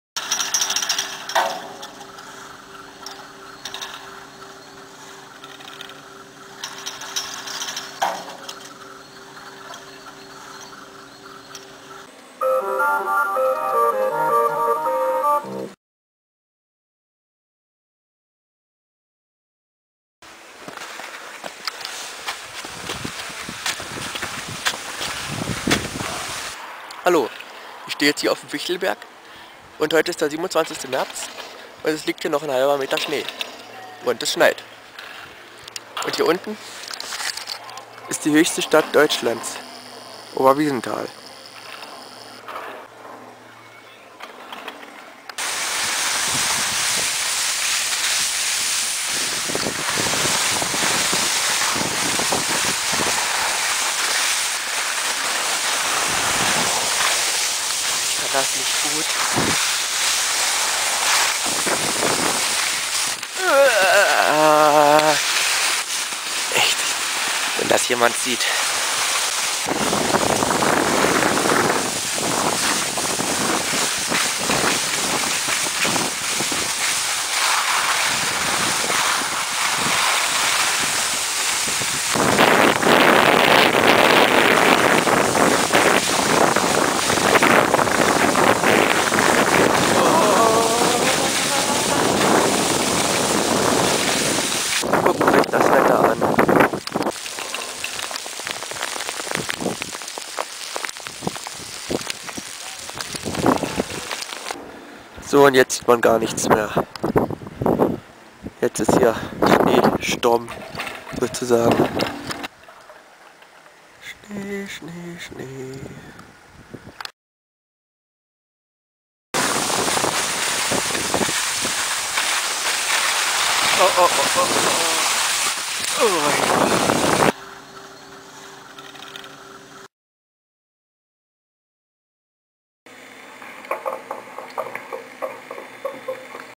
Ski fahren am Fichtelberg 27.3.2015
Ich war in Oberwiesenthal zum Ski fahren :-)